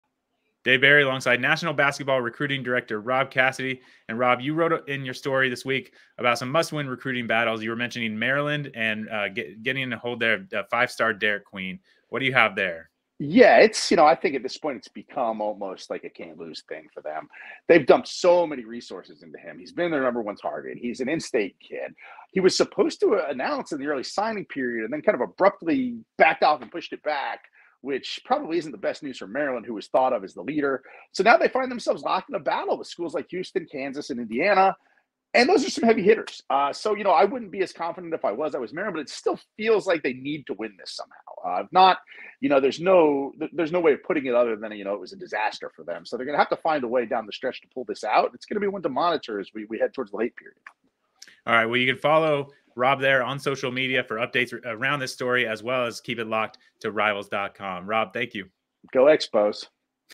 Must-win for Maryland: five-star center Derik Queen
Rob Cassidy joins Dave Berry to discuss what he calls a must-win battle for the Terrapins, 2024 five-star Derik Queen.